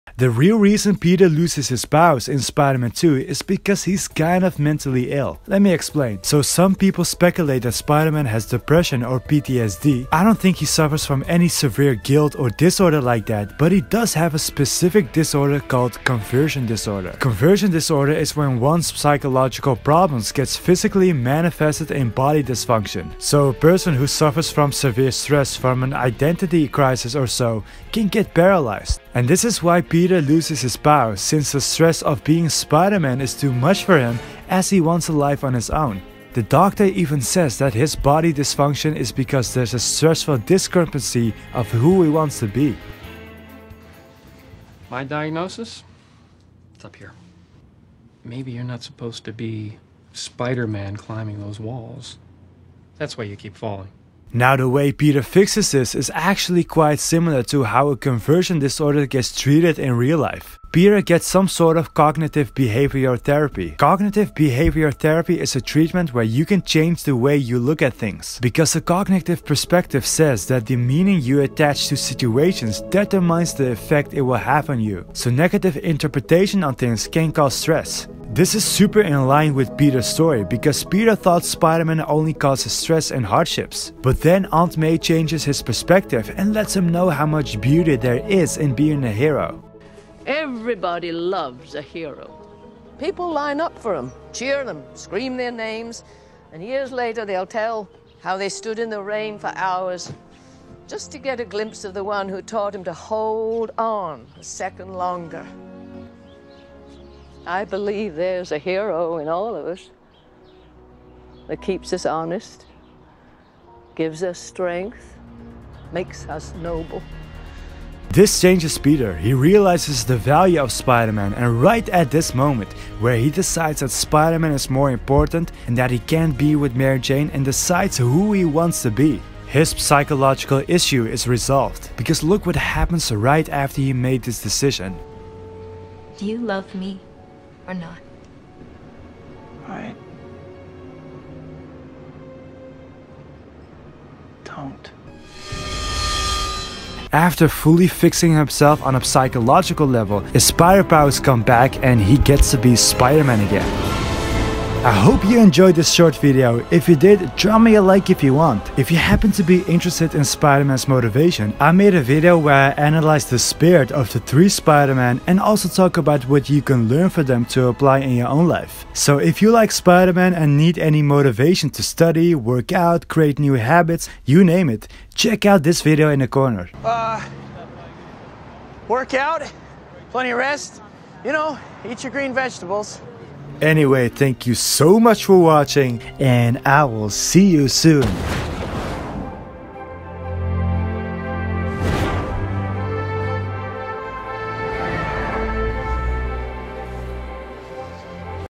The Psychology of SPIDER-MAN 2
There’s more to Peter losing his powers because of him not wanting to be Spider-Man. In this video I talk about the exact reason Peter loses his powers and discuss whether he has a disorder or not.